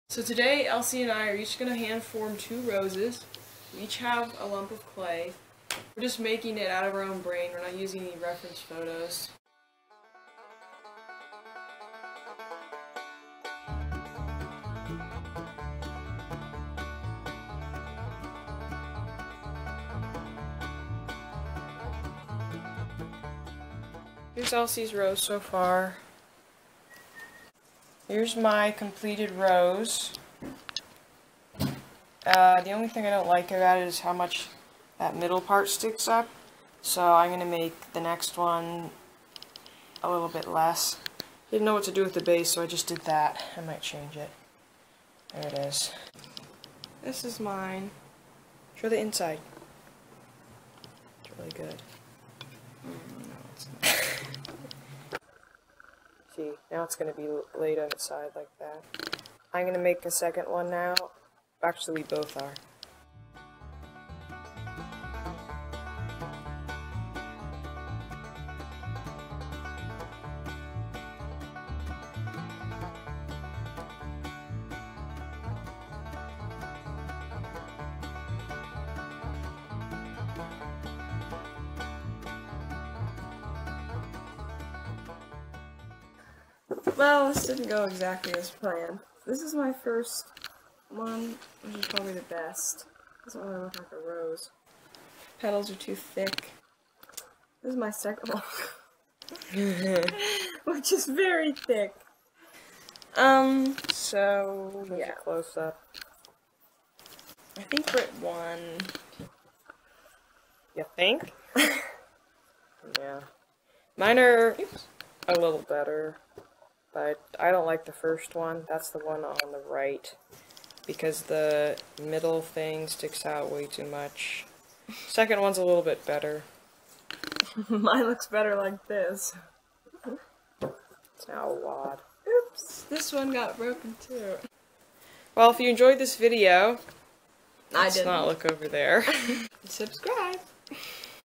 Hand-Forming Roses
In this video, B and E each hand-form 2 roses. Some turn out better than others :-]